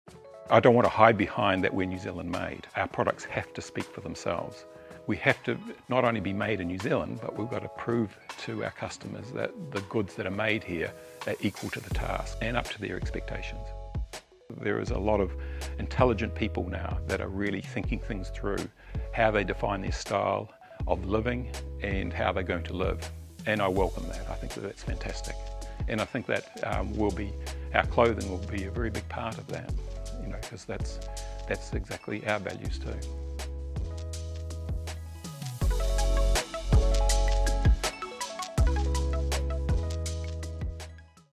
Creating sustainable clothing in New Zealand | Earth Sea Sky
INCLUDED IN THIS EPISODE
Earth Sea Sky was formed in December 1990 when David teamed up with clothing designer, Sydney Mulligan. Since then, most of New Zealand’s outdoor clothing brands have taken their production offshore. Earth Sea Sky is committed to remaining New Zealand Made. The Ellis family believes this is vital in maintaining the brand’s consistent quality and uniqueness.

GET FEATURED ON A KIWI ORIGINAL

MAIL
3-11 Hunter Street | Wellington | New Zealand.